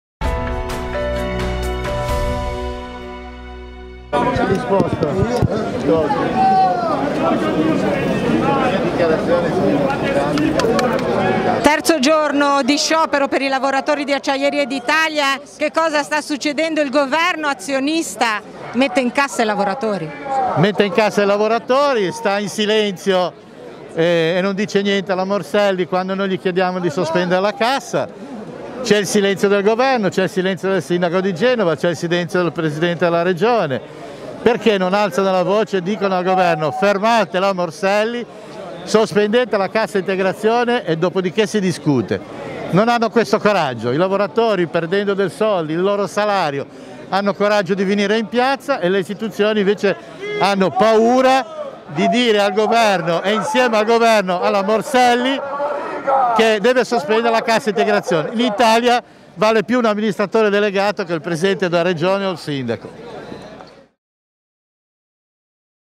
Bruno Manganaro - FIOM
Corteo ex-Ilva, oggi una delle giornate più calde della protesta.
Operai manganellati davanti alla Prefettura e un funzionario della Digos in pronto soccorso.